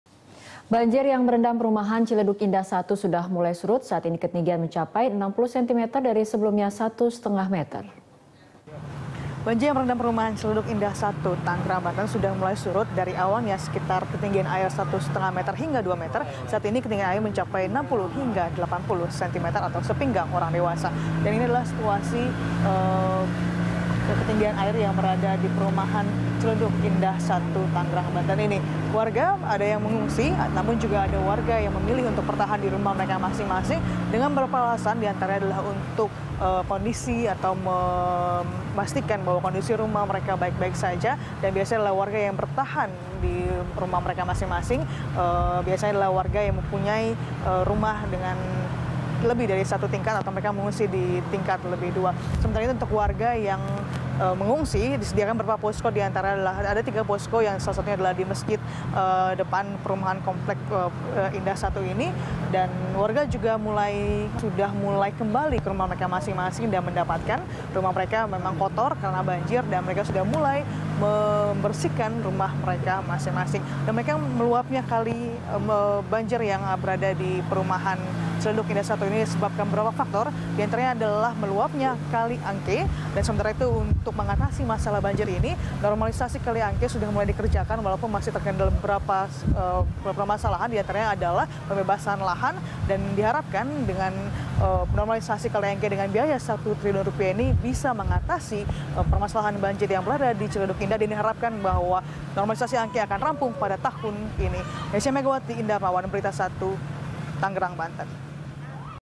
Banjir Ciledug Indah Surut Jadi 60 Centimeter
Banjir yang merendam kawasan Ciledug Indah, Tangerang Selatan, Banten, Selasa (14/1/2014), mulai surut. Ketinggian air sebelumnya mencapai 1,5 meter kini menjadi 60-80 centimeter.

Facebook.com/BeritaSatuTV
Youtube.com/BeritaSatu